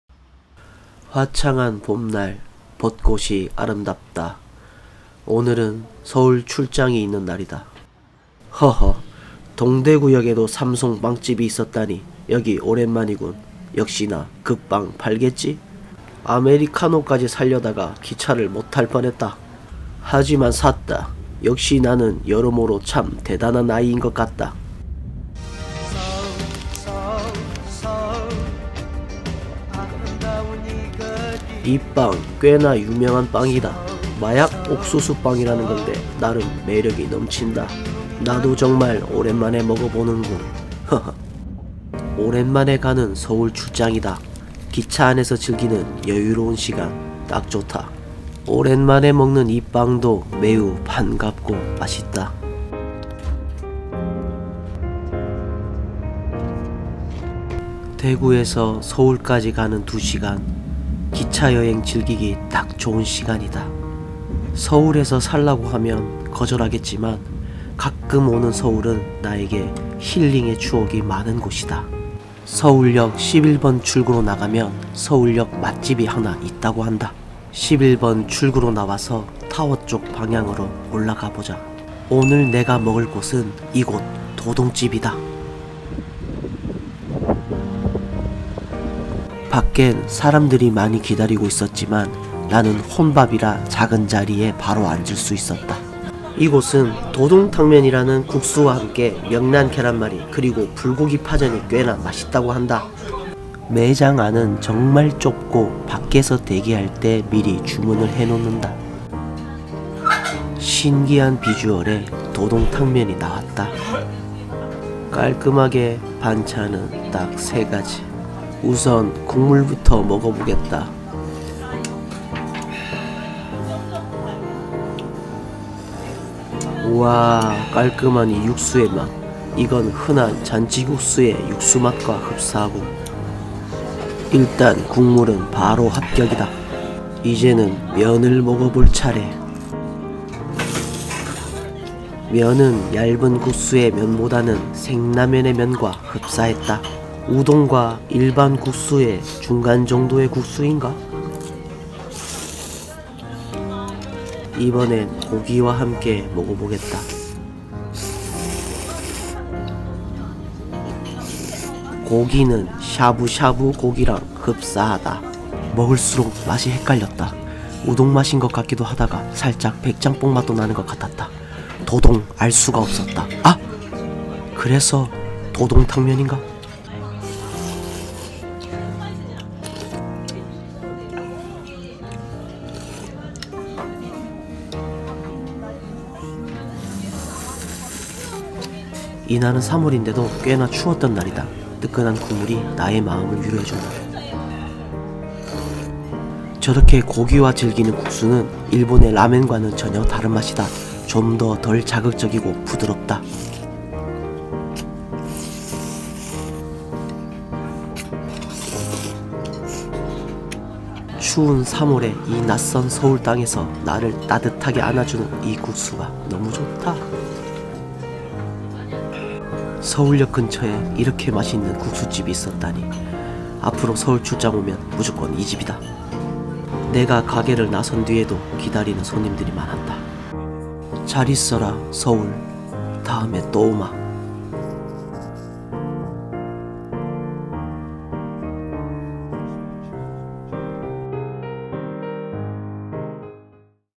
[서울맛집]서울역_도동집(도동탕면)
서울출장때 가본 집입니다!
서울역에서 식사하실 때 참고하세요~!

서울 용산구 후암로 80
지번:후암동 439-13 1층

월~토요일 11:30 - 22:00
BreakTime 15:00~17:00

공휴일 11:30 - 22:00
BreakTime 없음
일요일 휴무